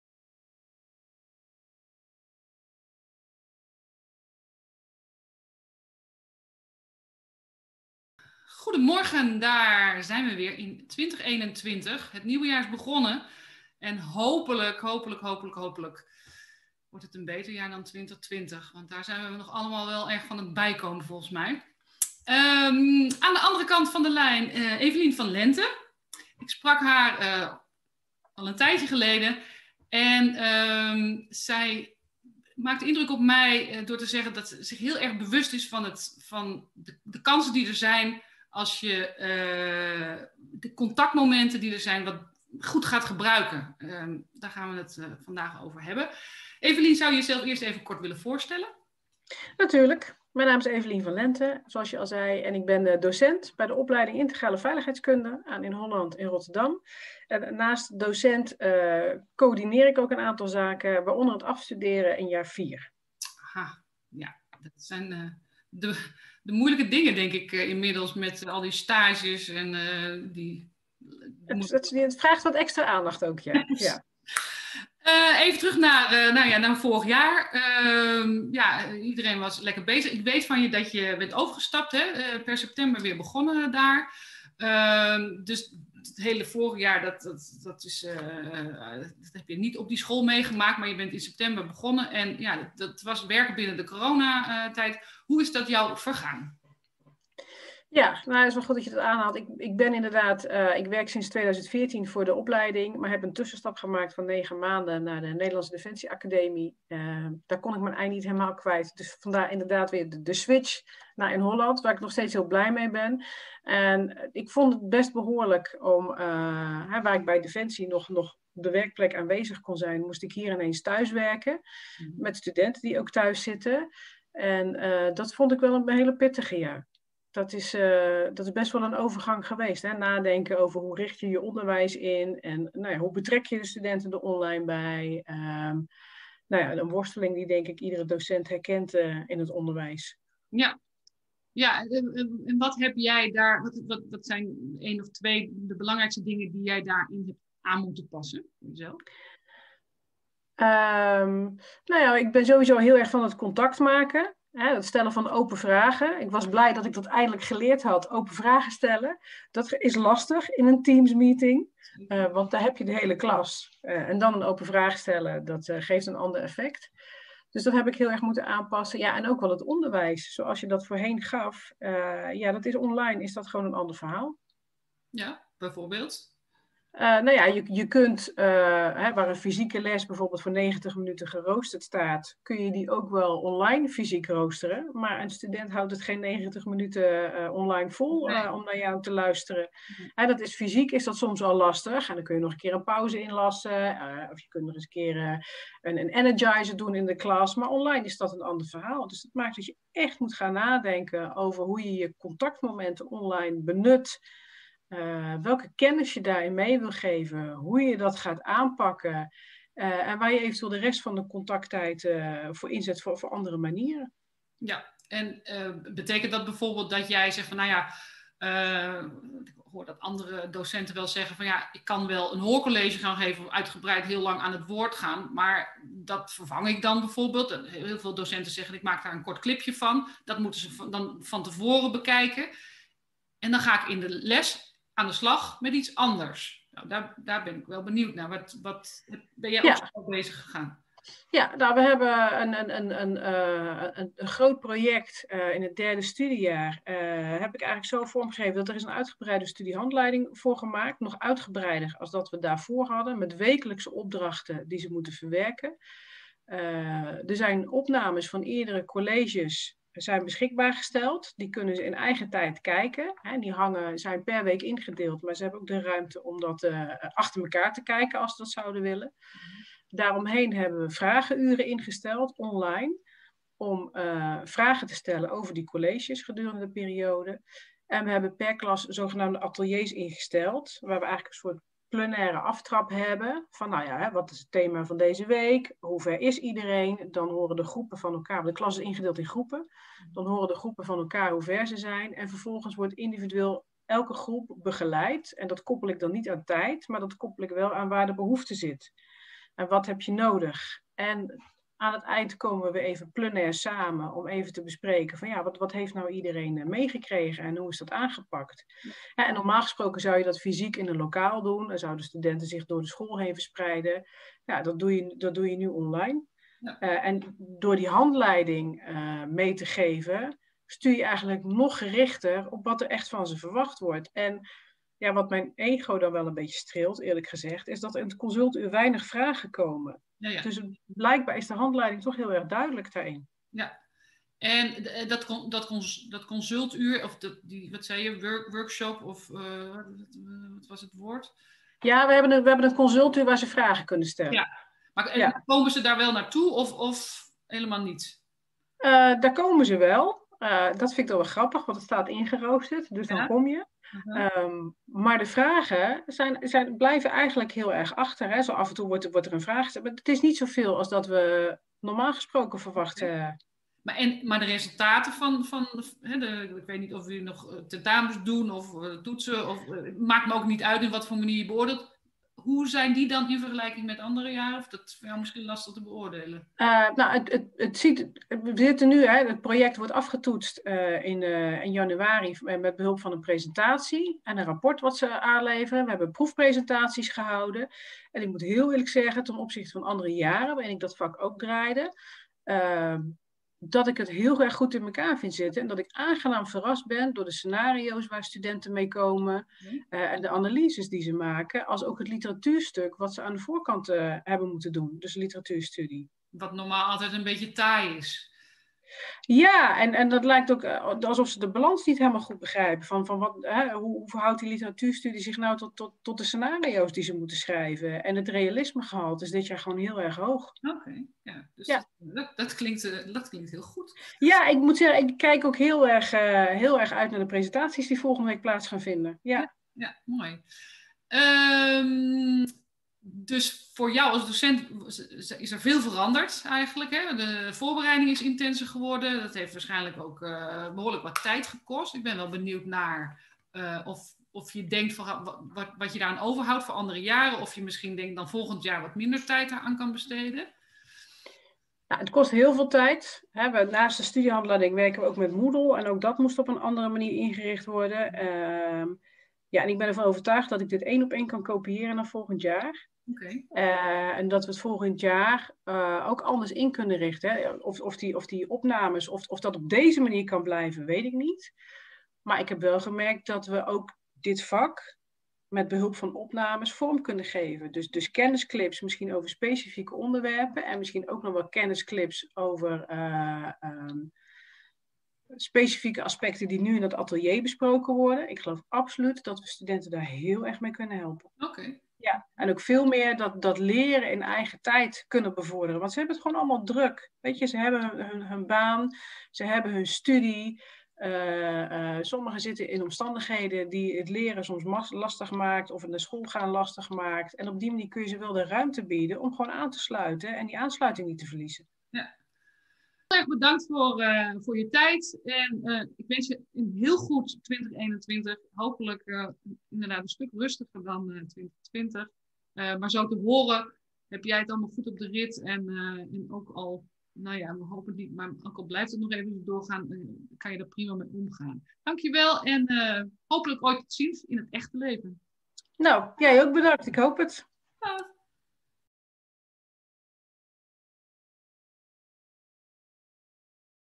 Koffie break met Evelien van Lenthe
Deze week in 'Koffie break met...': Evelien van Lenthe, docent bij de opleiding Integrale Veiligheidskunde aan de Hogeschool Inholland in Rotterdam en afstudeercoördinator van de vierdejaars. Ze is haar onderwijs volledig anders gaan vormgeven. Met opdrachten, vragenuren, ateliers en een andere studiehandleiding. De resultaten lijken veelbelovend! Link artikel